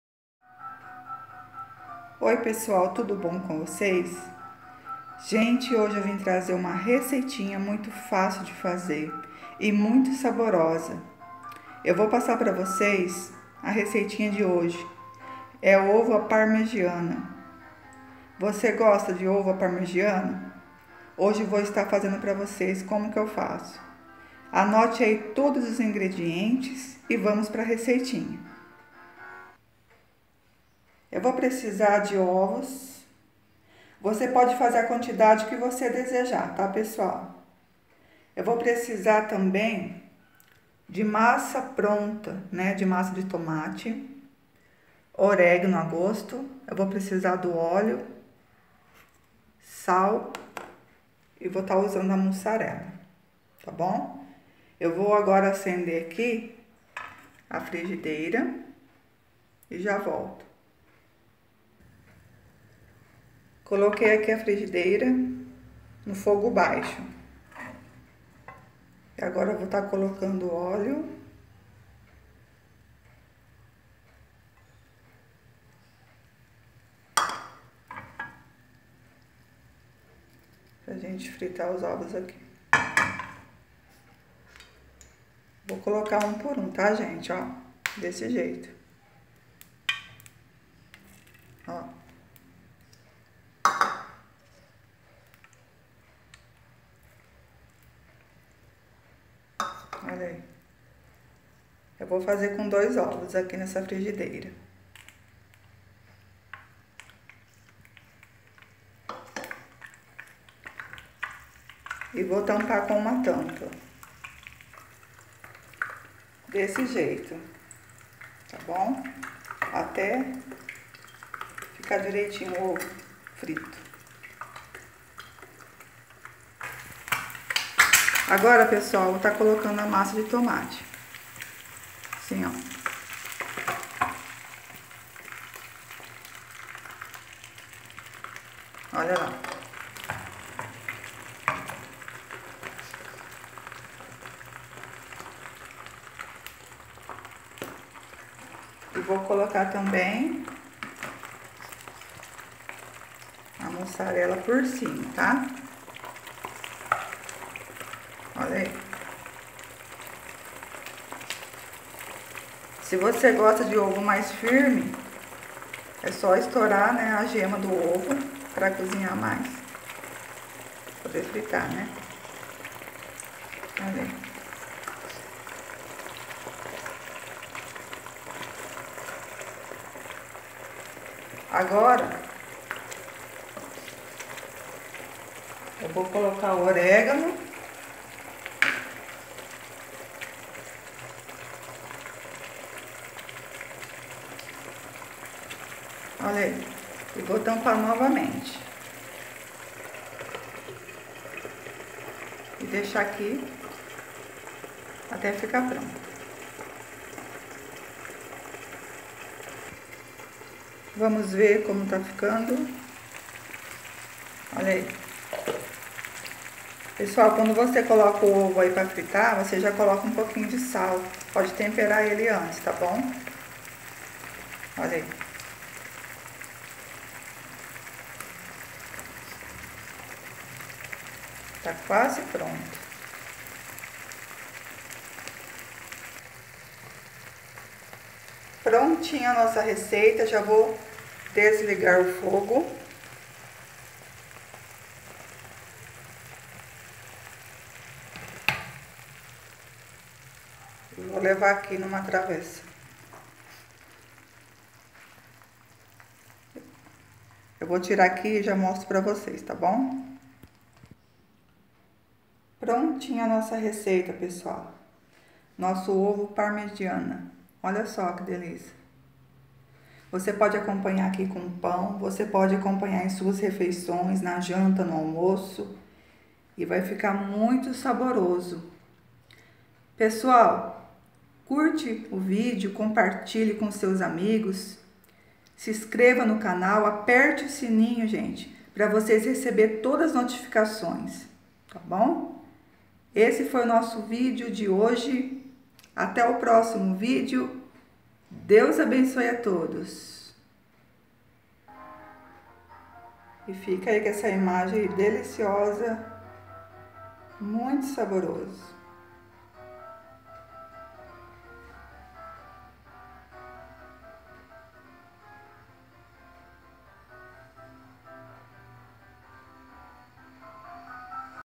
Como fazer ovo parmegiana / Daniela Napoleão
Ovo parmegiana feito na frigideira , muito fácil de fazer e fica delicioso !
ingredientes :
ovos
oleo
massa de tomate pronta
orégano a gosto
sal
mussarela